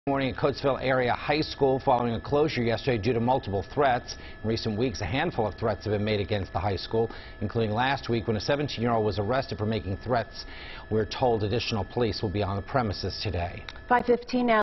Classes at Coatesville Area High School resume Friday
The school is reopening Friday following a closure due to two shooting threats.